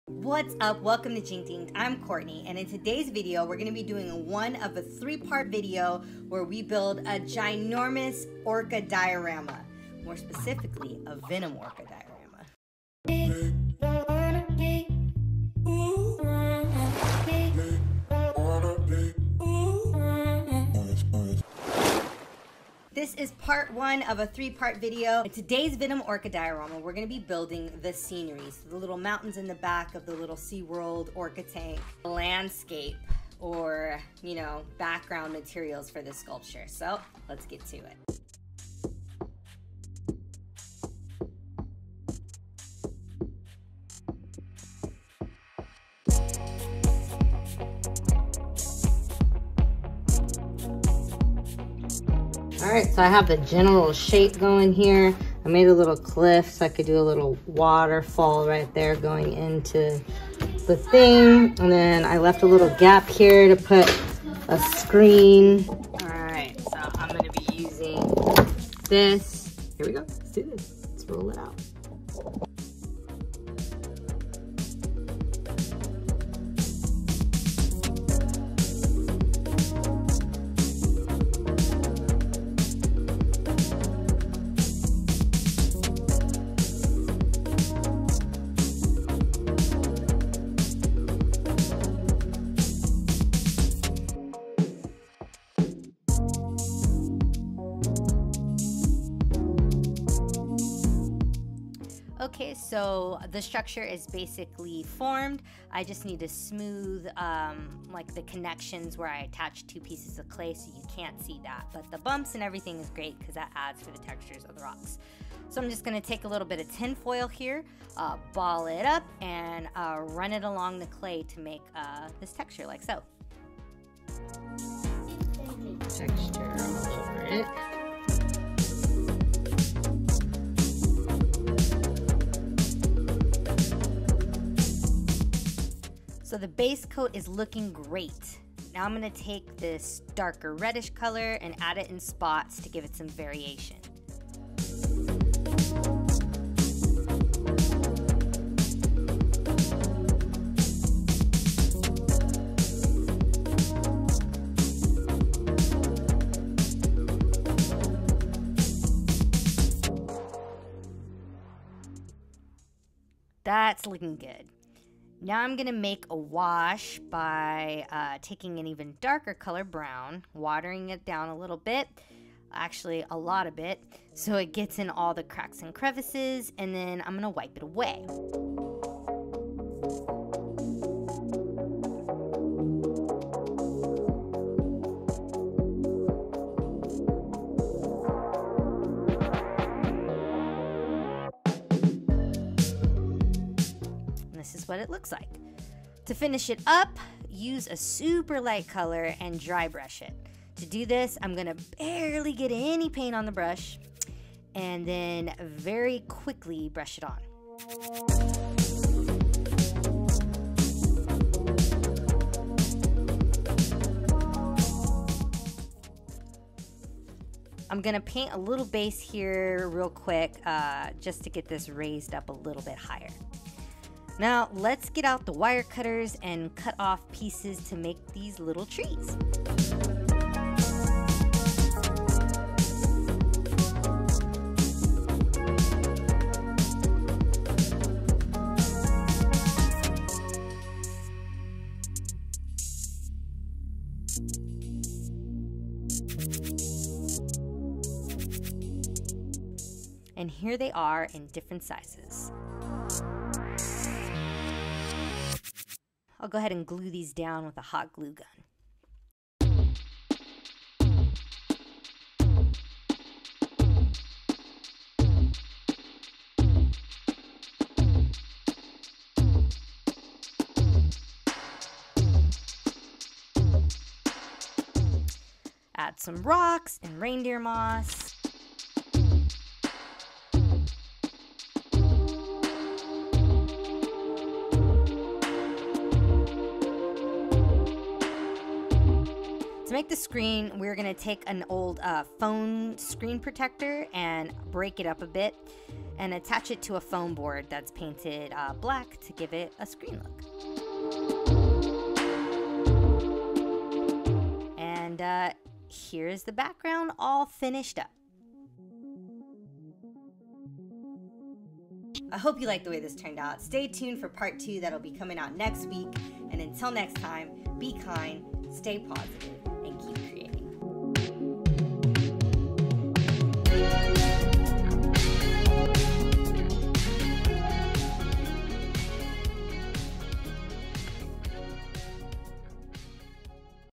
Venom Orca Diorama Sculpt With Polymer Clay Part 1
Hey guys!

This is part 1 of a 3 part build. In today’s video, I’ll be sculpting and building the background scenery.

We are always trying to find fun and creative ways to be expressive with art!